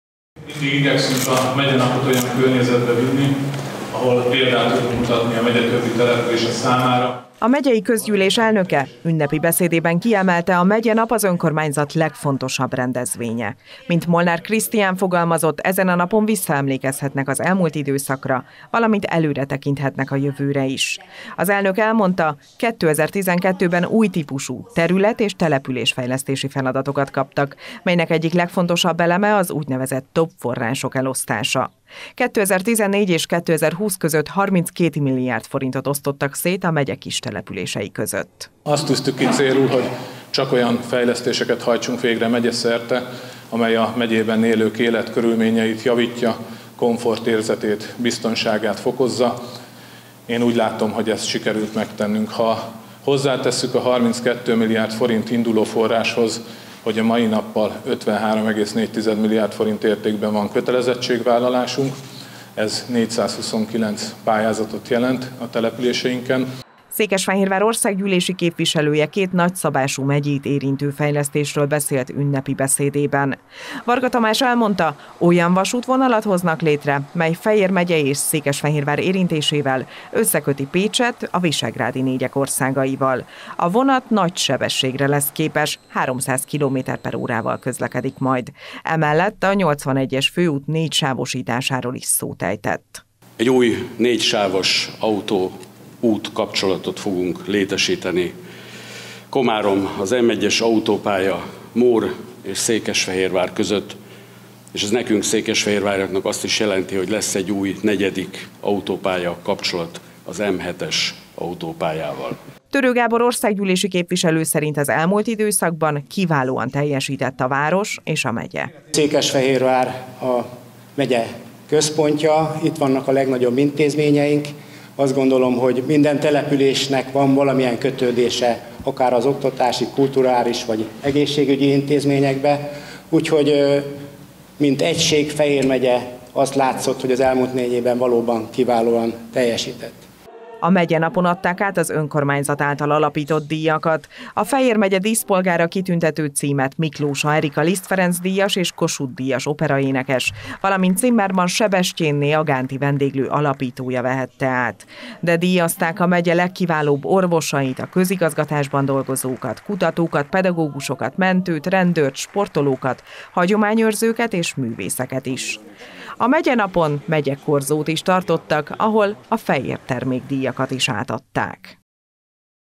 MEGYENAP SZÉKESFEHÉRVÁR
Ünnepi közgyűléssel kezdődött el a Megyenap pénteken Székesfehérváron. A rendezvényen átadták a megyei közgyűlés kitüntetéseit, megyekorzó várta az érdeklődőket és átadták a „Fejér Termék 2022” elismerést is.